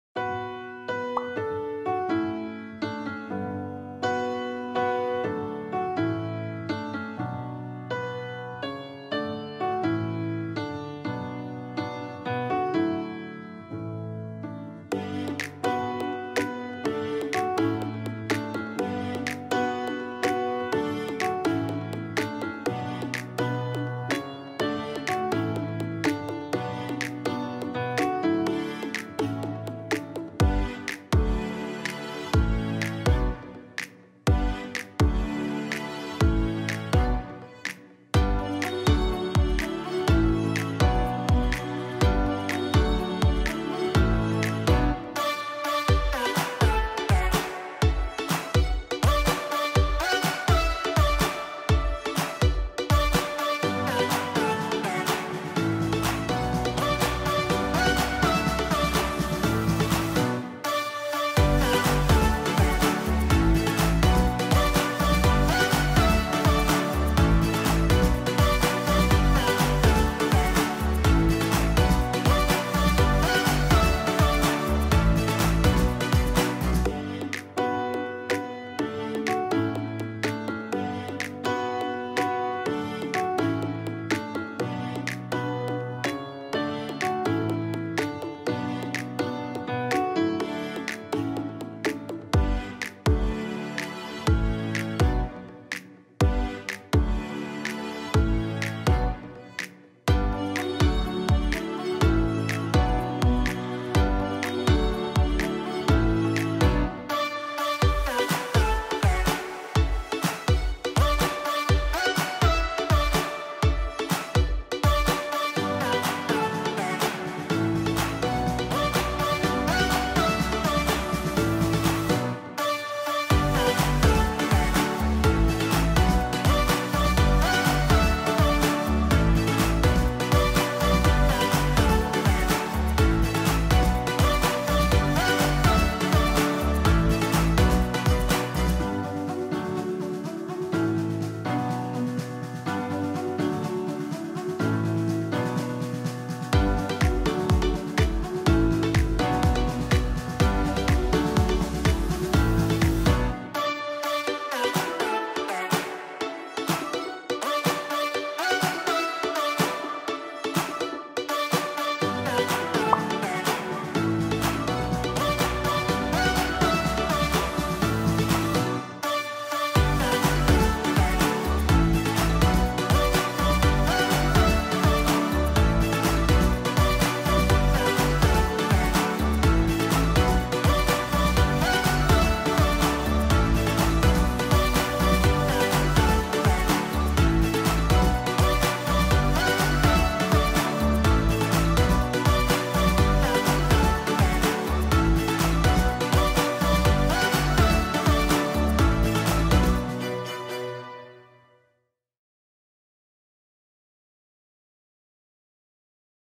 How to Make Iphone 15 Pro Notebook No Glue | Paper Craft Ideas
In this video, we'll be showing you how to make iphone 15 pro notebook. Paper craft ideas.

If you liked our video - how to make iphone 15 pro notebook.